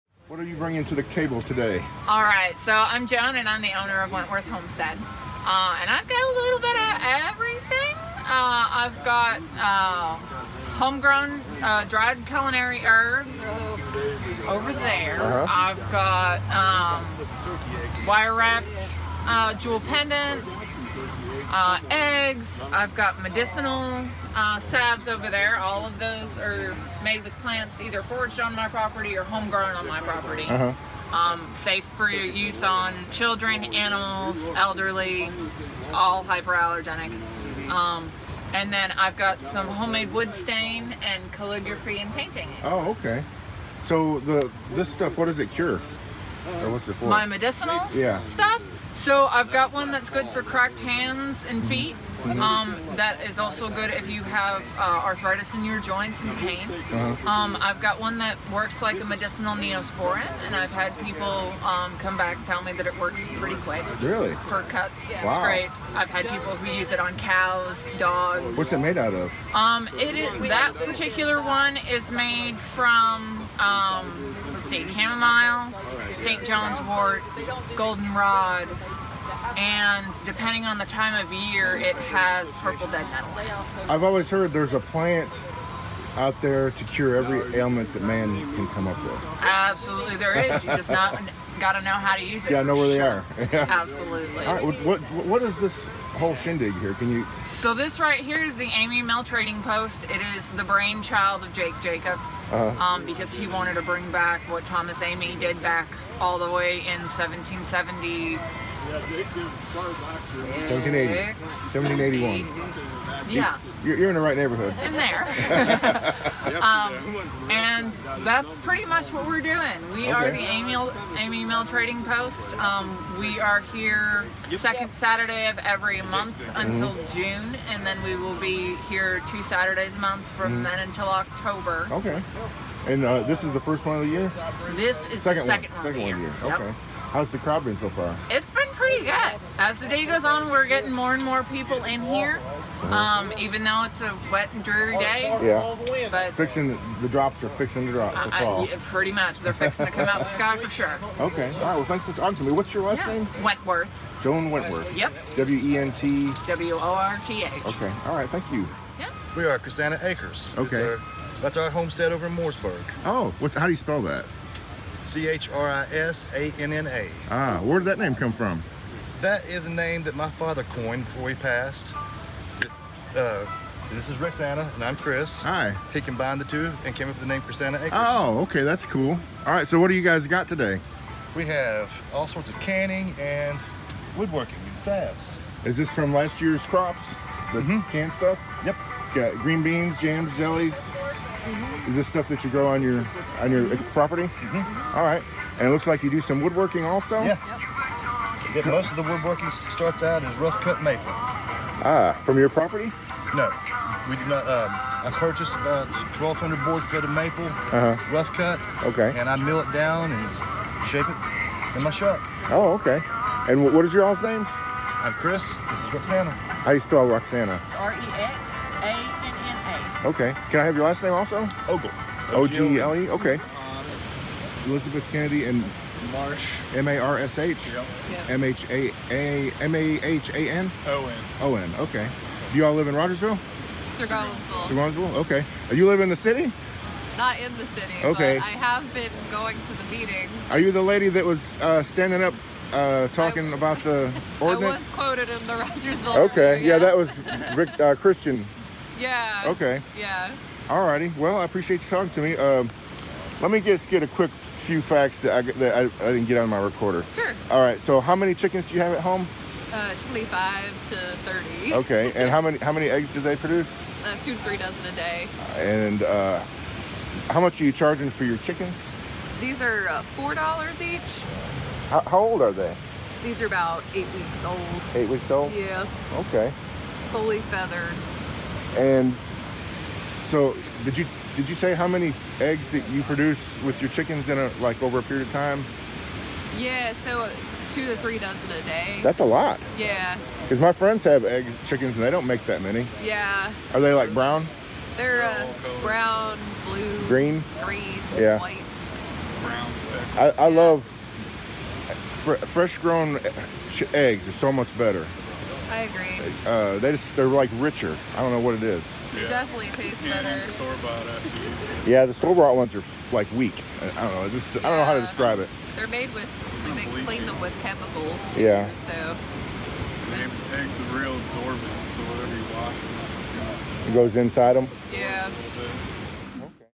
Amis Mill to host homesteader Trading Posts through October
Rogersville, TN